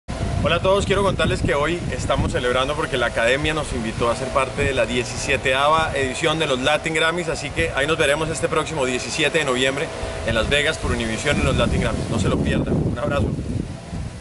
Fonseca - ¡Nos fuimos para los Latin Grammy's en Las Vegas!
Y nos fuimos a tocar a los Latin Grammy's en Las Vegas este próximo 17 de Noviembre. No se lo pierdan, estén pendientes saldrá en exclusiva por Univision. ¡Gracias a todos!